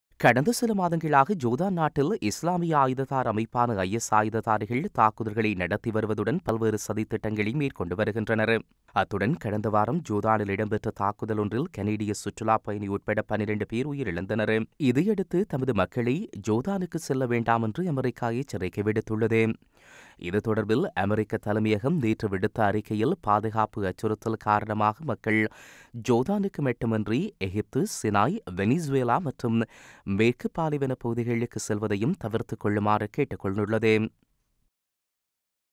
ஜோர்தானுக்கு பயணத்தைத் தவிர்த்துக் கொள்ளுமாறு அமெரிக்கா பயண எச்சரிக்கை
ஜோர்தானில் ஆயுததாரிகளின் அச்சுறுத்தல்களை அடுத்து அந்நாட்டுக்கான பயணத்தைத் தவிர்த்துக் கொள்ளுமாறு பொதுமக்களுக்கு அமெரிக்க அரசாங்கம் பயண எச்சரிக்கை விடுத்துள்ளது.